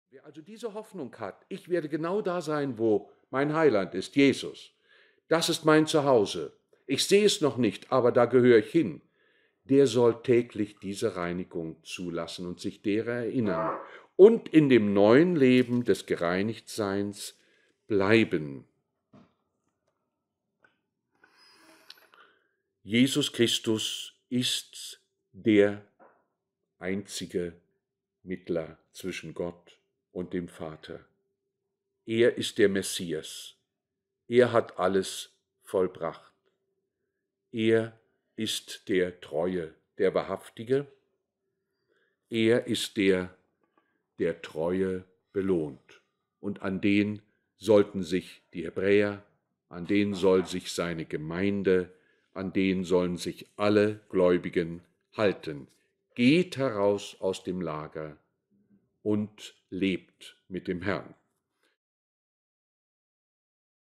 2010.07.12.a (4 / 4) - (Hebräer 13, 10-14) - Heilige Absonderung zum wahren Gottesdienst
Bibelfreizeit 2010 in 90617 Puschendorf; Generalthema: "Die Hoffnung der Gemeinde Jesu und Israels" in 17 Teilen; Predigtthema: 07 "(Hebräer 13, 10-14) - Heilige Absonderung zum wahren Gottesdienst"; Veranstalter: Evangelische Berliner Schriften-Mission, Ruhlebener Str. 9, 13597 Berlin; Prediger: Wolfgang Schmidt - Teilupload 4 von 4